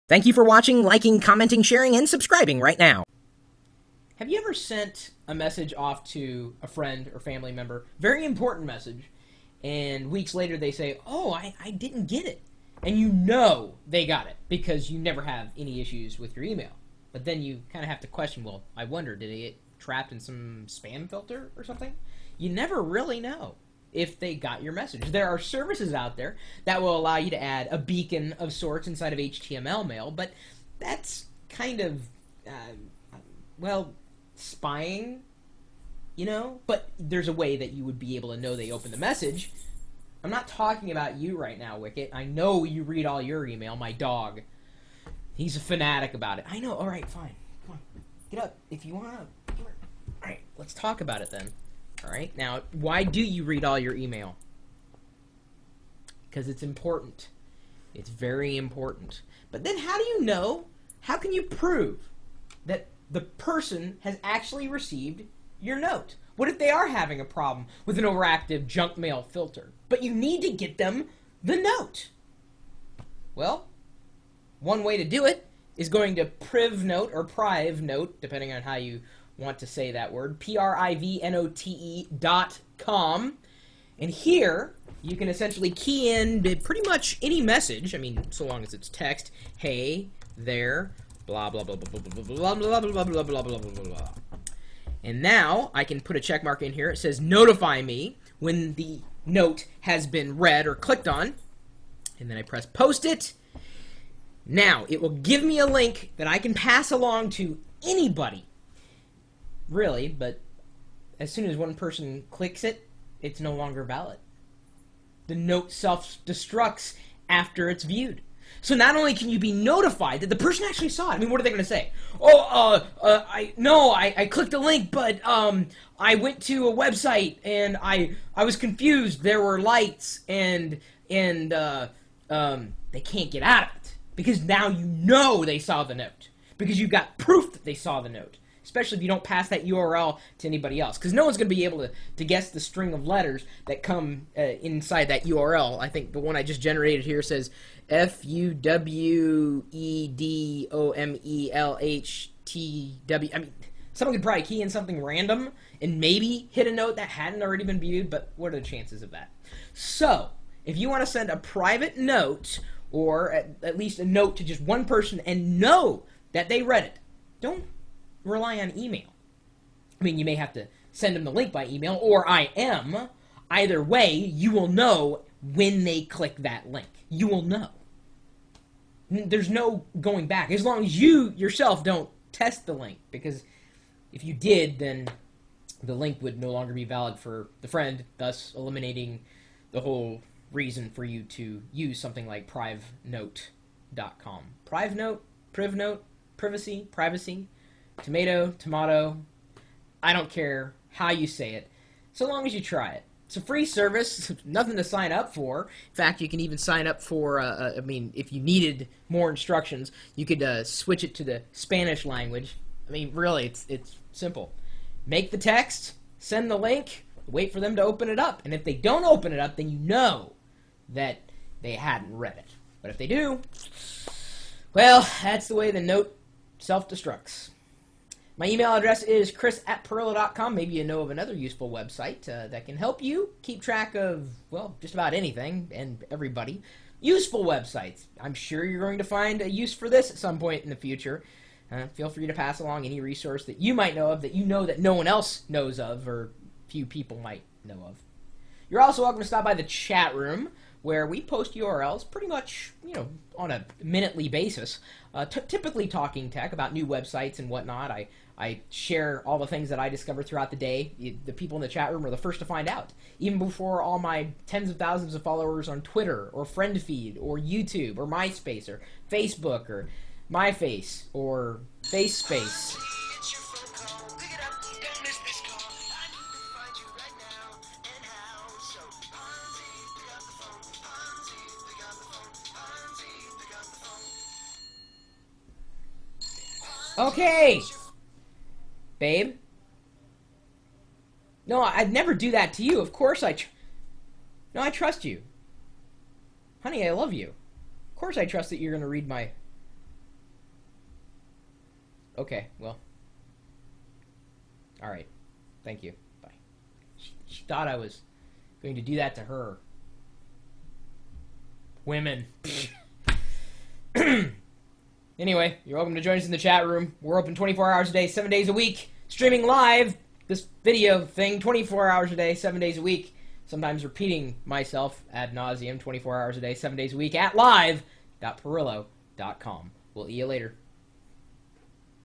How do you Know your Friend Saw your Note?
- Have you ever sent an important message, only to have the recipient later say they never received it? You're pretty sure that they got it an ignored it, but you can't prove it. Isn't it great that there's a way you can finally know once and for all whether your message was received or not?